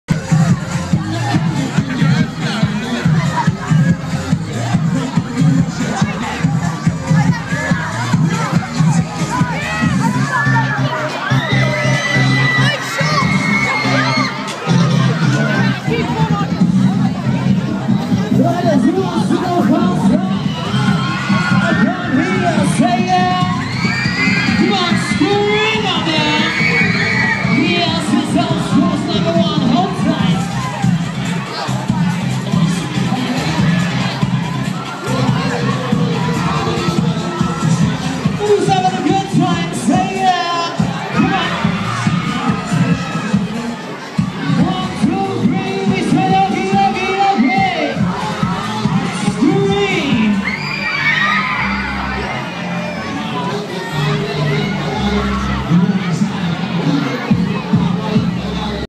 Spin Ball Roundup Didcot 2012
Independent leisure's
Spin Ball Cage ride at
Didcot Christmas fair